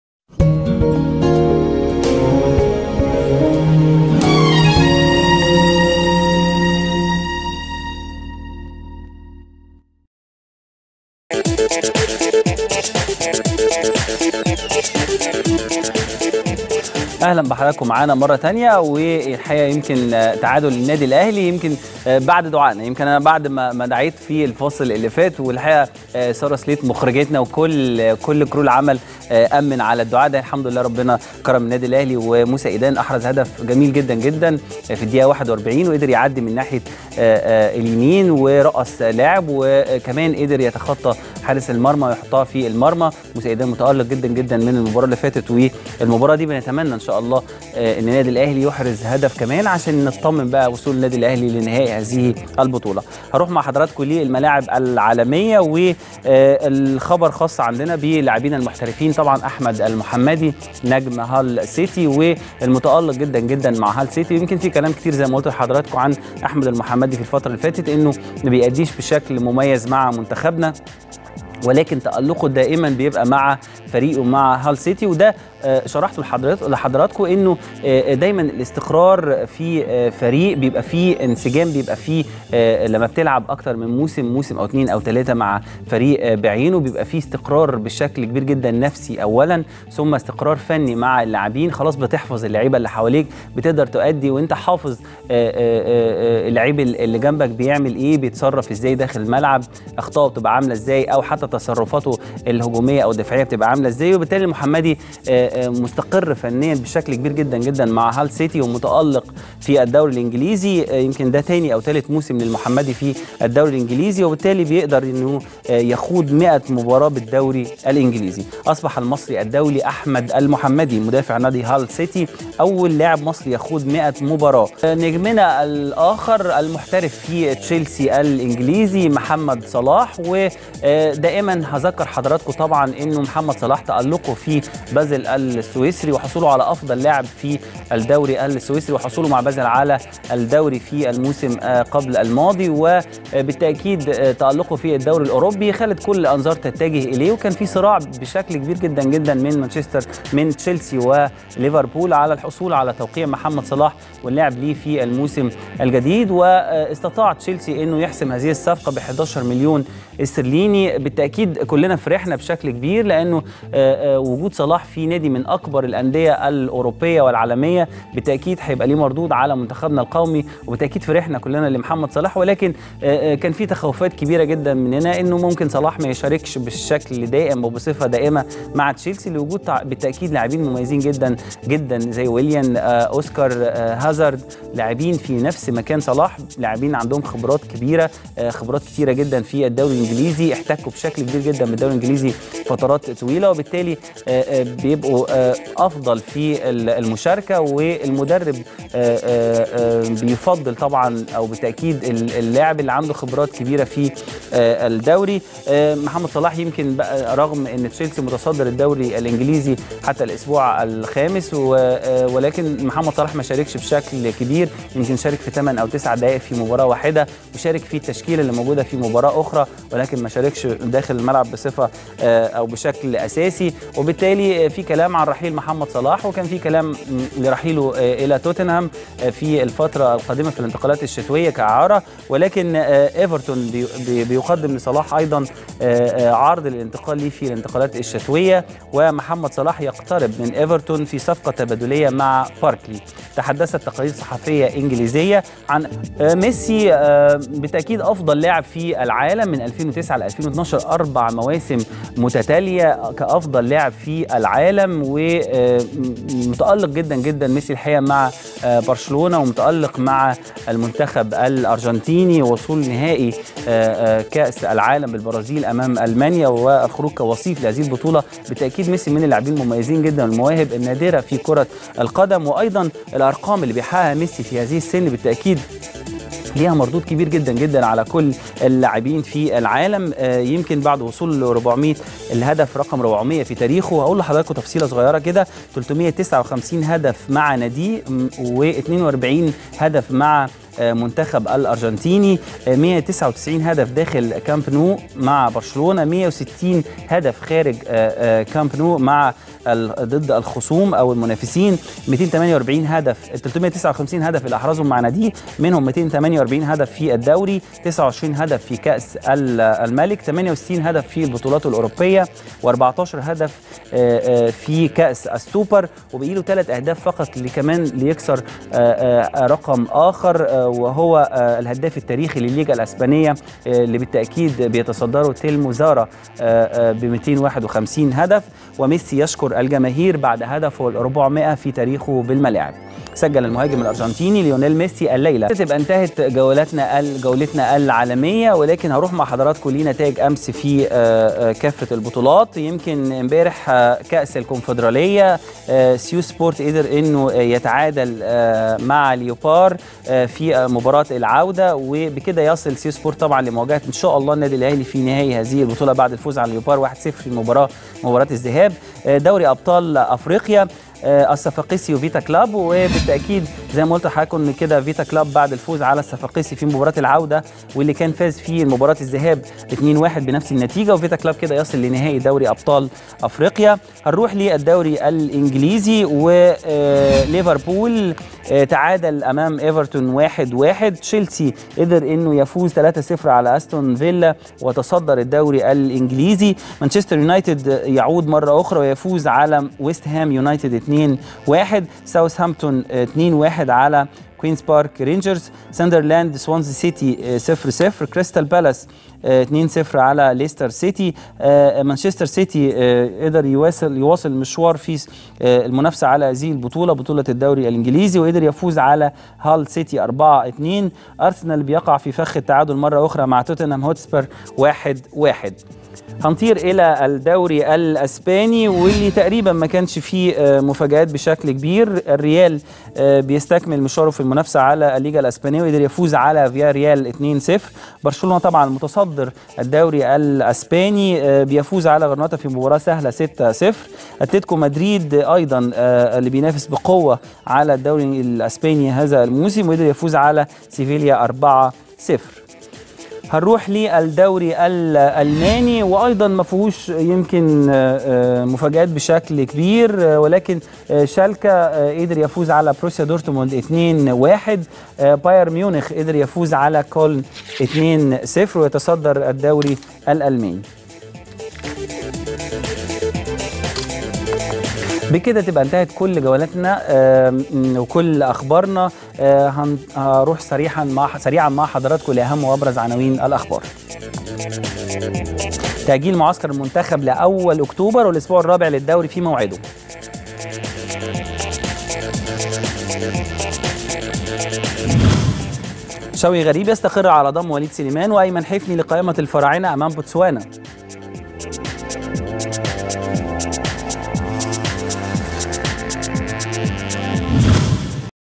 النهار News | الاخبار العالمية| المحمدى أول مصرى يخوض 100 مبارة بالدورى الانجليزى
محمد صلاح يقترب من ايفرتون فى صفقة تبادلية مع باركلى

ريو أفى يسقط أمام براجا فى غياب كوكا بالدورى البرتغالى

يينيتنز يقترب من العودة للدورى الانجليزى

ميسى يشكر الجماهير بعدهدفة رقم 400 فى تاريخة بالملاعب